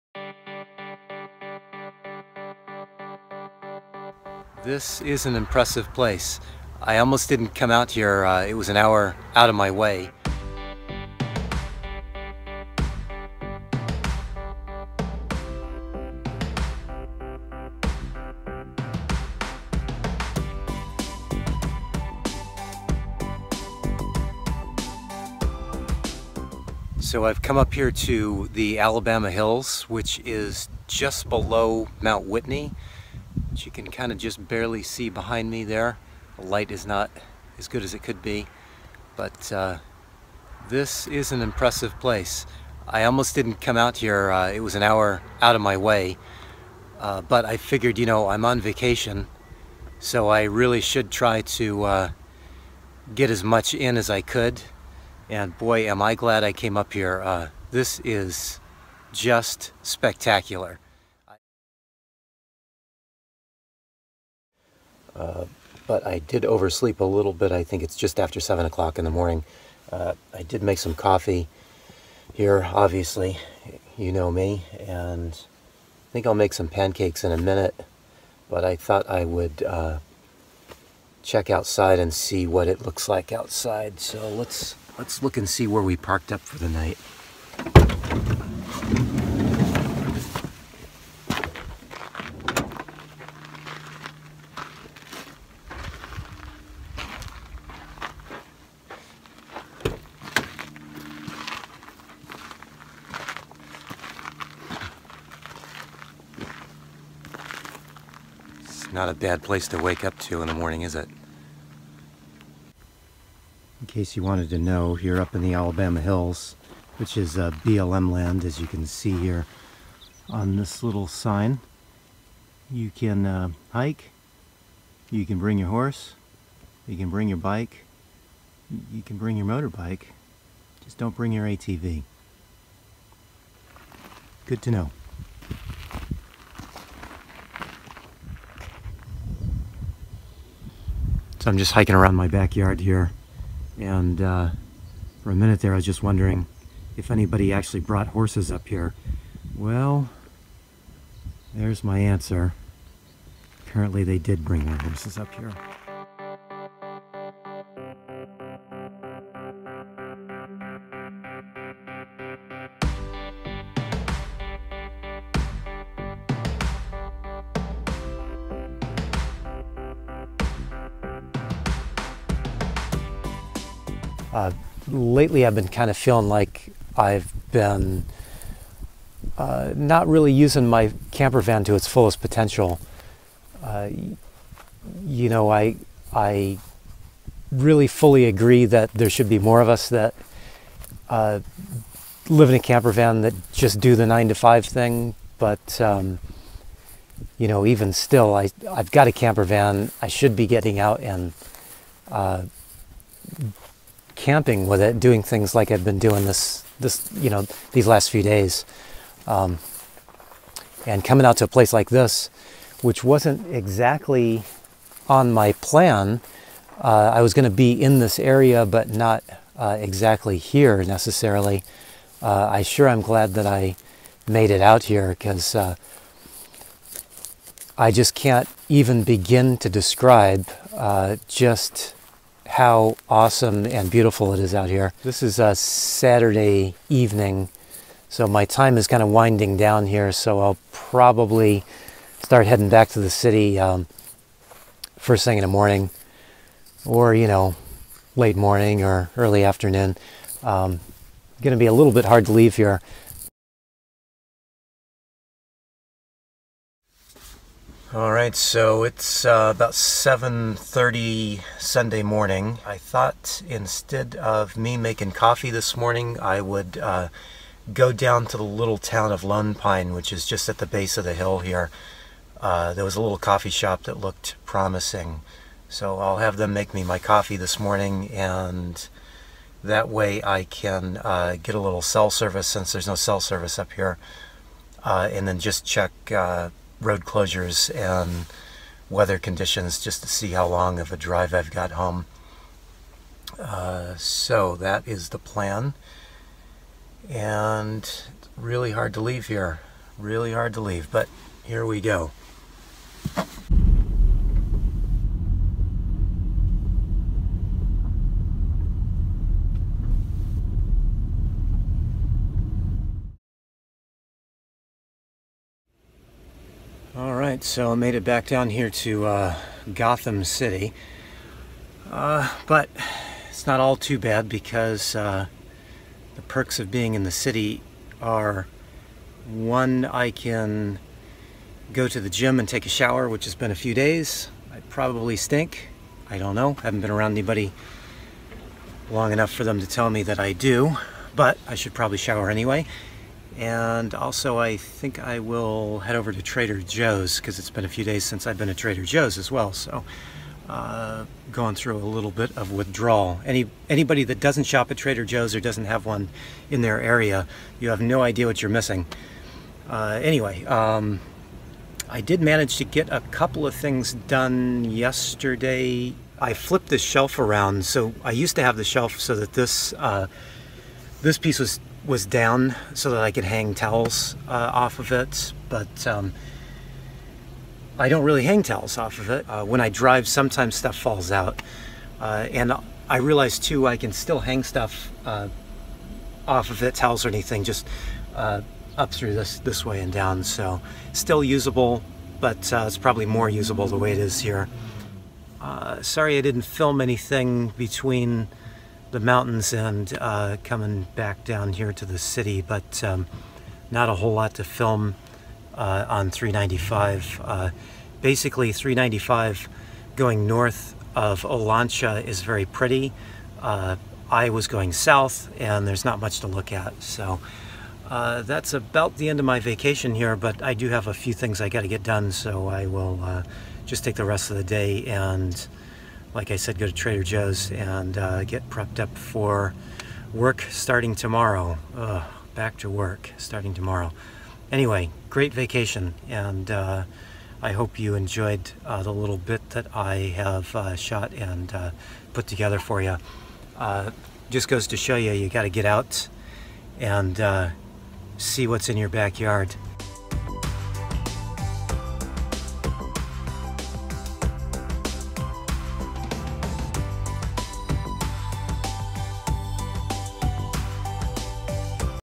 One Year Van Anniversary part 3 (or the most spectacular place I've ever been)
This was the final destination of my one year van anniversary. This area, Alabama Hills in the Eastern Sierras, is now my favorite place I've ever visited.
Thank you to all who have watched and encouraged me over these last few months.